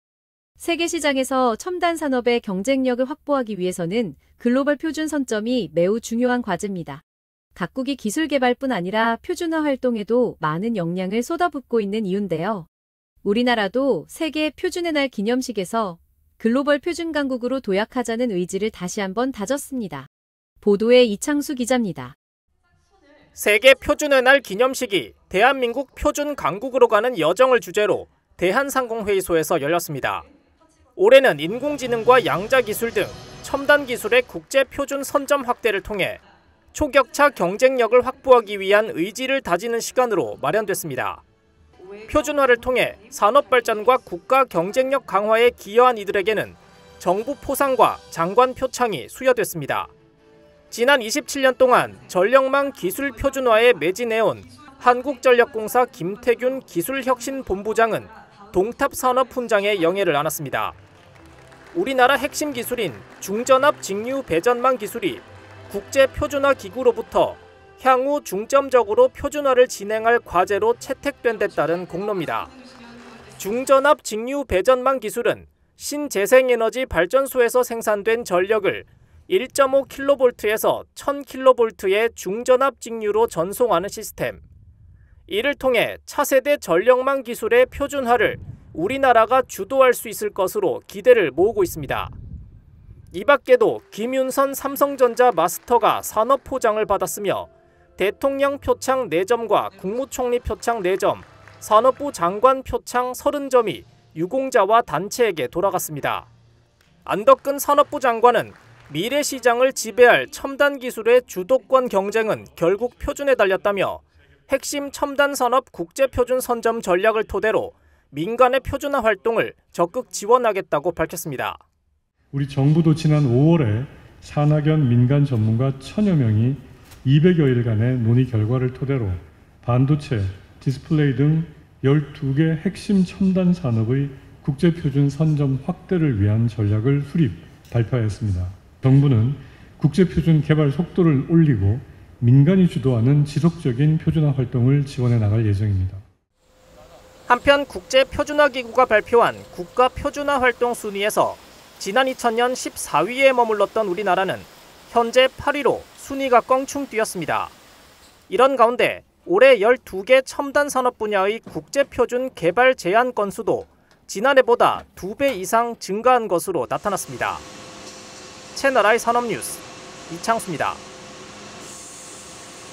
세계 표준의 날…첨단기술 글로벌 표준 강국 도약_산업뉴스 [산업방송 채널i]
[앵커멘트]
세계시장에서 첨단산업의 경쟁력을 확보하기 위해서는 글로벌 표준 선점이 매우 중요한 과제입니다. 각국이 기술개발뿐 아니라 표준화 활동에도 많은 역량을 쏟아붓고 있는 이유인데요. 우리나라도 세계 표준의 날 기념식에서 글로벌 표준 강국으로 도약하자는 의지를 다시 한번 다졌습니다. 보도에 이창수 기자입니다.

[리포트]
세계 표준의 날 기념식이 ‘대한민국, 표준강국으로 가는 여정’을 주제로 대한상공회의소에서 열렸습니다.

올해는 인공지능과 양자기술 등 첨단기술의 국제표준 선점 확대를 통해 초격차 경쟁력을 확보하기 위한 의지를 다지는 시간으로 마련됐습니다.

표준화를 통해 산업 발전과 국가 경쟁력 강화에 기여한 이들에게는 정부포상과 장관 표창이 수여됐습니다.

지난 27년 동안 전력망 기술 표준화에 매진해온 한국전력공사 김태균 기술혁신본부장은 동탑산업훈장의 영예를 안았습니다.

우리나라 핵심 기술인 ‘중전압직류 배전망 기술’이 국제표준화기구로부터 향후 중점적으로 표준화를 진행할 과제로 채택된 데 따른 공로입니다.
중전압직류 배전망 기술은 신재생에너지 발전소에서 생산된 전력을 1.5킬로볼트에서 1,000킬로볼트의 중전압 직류로 전송하는 시스템.

이를 통해 차세대 전력망 기술의 표준화를 우리나라가 주도할 수 있을 것으로 기대를 모으고 있습니다.

이밖에도 김윤선 삼성전자 마스터가 산업포장을 받았으며, 대통령 표창 4점과 국무총리 표창 4점, 산업부 장관 표창 30점이 유공자와 단체에게 돌아갔습니다.

안덕근 산업부 장관은 미래 시장을 지배할 첨단기술의 주도권 경쟁은 결국 표준에 달렸다며, 핵심 첨단산업 국제표준 선점 전략을 토대로 민간의 표준화 활동을 적극 지원하겠다고 밝혔습니다.

[현장음 – 안덕근 / 산업통상자원부 장관]
3:14 지난 5월 산학연 민간 전문가 1천 여명이 200여 일 간의 논의 결과를 토대로 반도체 디스플레이 등 12개 핵심 첨단산업의 국제표준 선점 확대를 위한 전략을 수립해 발표했습니다. 3:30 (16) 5:23 정부는 국제표준 개발 속도를 올리고 민간이 주도하는 지속적인 표준화 활동을 지원해 나갈 예정입니다. 5:30 (7)

한편, 국제표준화기구가 발표한 국가 표준화 활동 순위에서 지난 2000년 14위에 머물렀던 우리나라는 현재 8위로 순위가 껑충 뛰었습니다.

이런 가운데, 올해 12개 첨단산업 분야의 국제표준 개발·제안 건수도 지난해보다 두 배 이상 증가한 것으로 나타났습니다.

채널i 산업뉴스 이창수입니다.